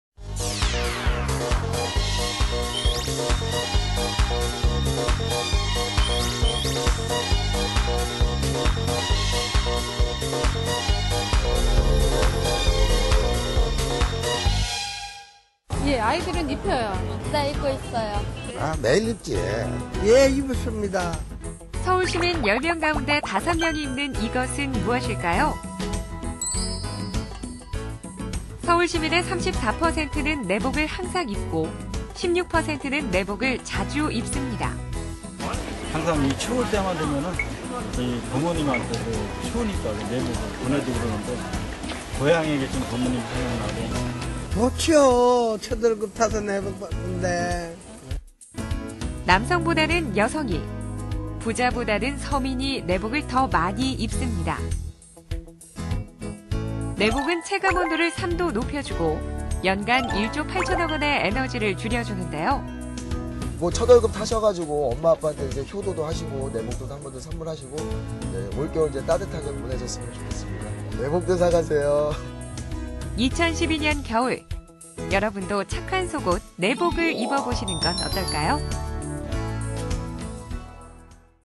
숫자로 보는 서울 2회 내복
서울 시민 10명 가운데 5명이 입는 이것은 무엇일까요?
날씨가 추워지면서  내복을 찾는 사람들이 늘어나고 있는데 몇%의 시민들이 찾고 있는지
내복의 효과는 어떤지 알아볼수 있습니다.
에너지까지 줄일수 있는 내복착용에 대해 알아볼까요